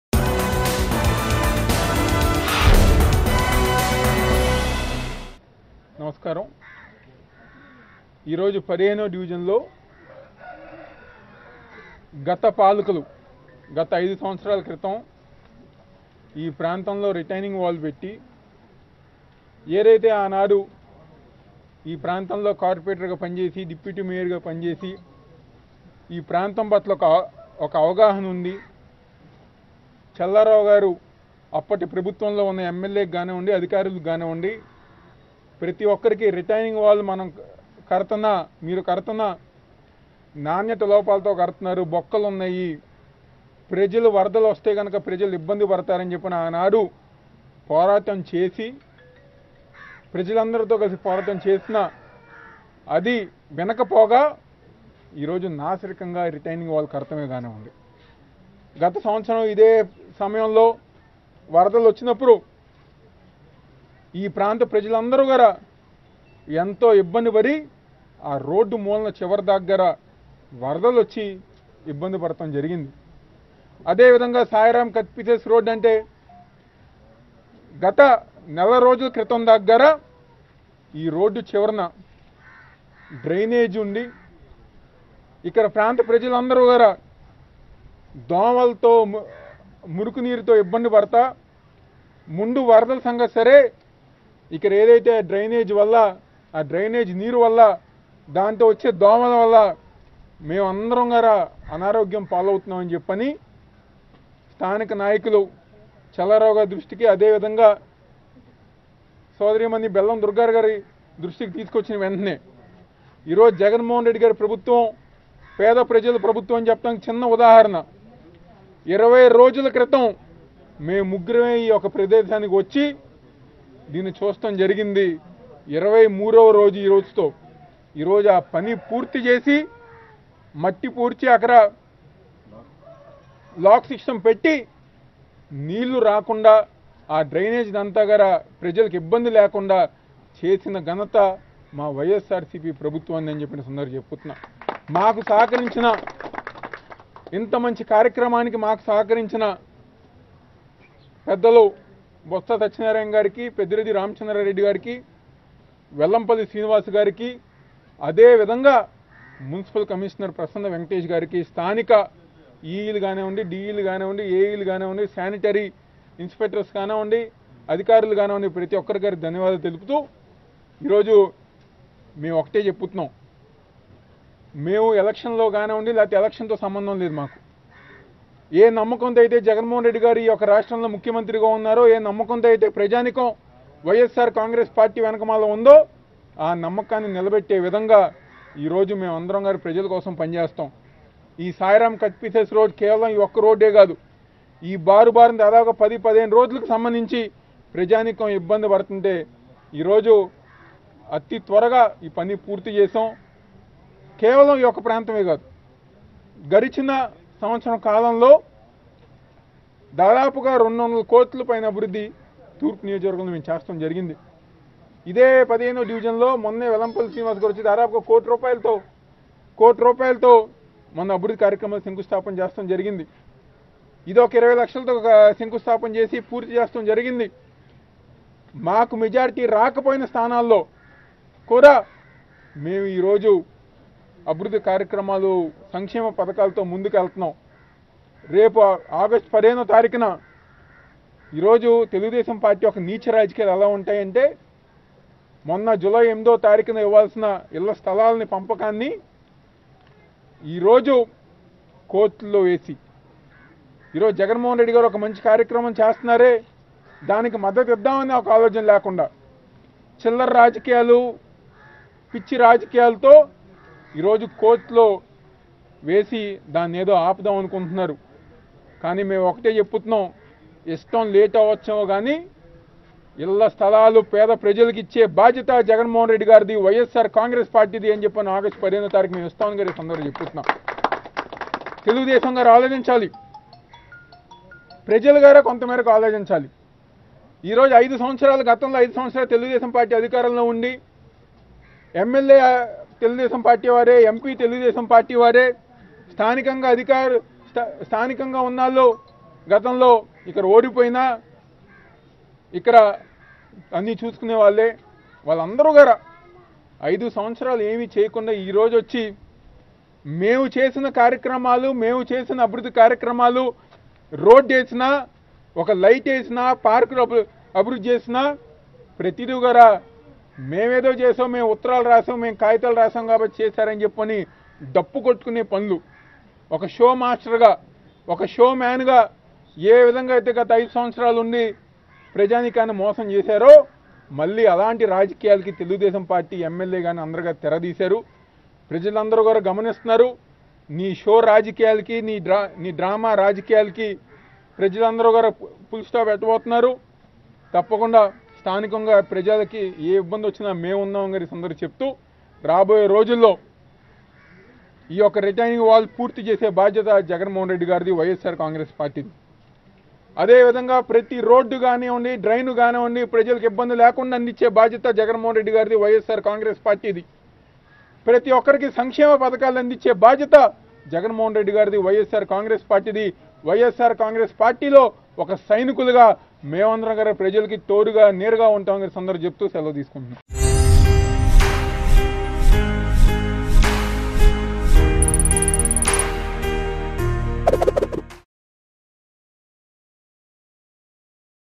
YCP Leader Devineni Avinash Sensational Comments On TDP Over Cheap Politics | AP News | Mango News
YCP Leader Devineni Avinash Sensational Comments On TDP Over Cheap Politics.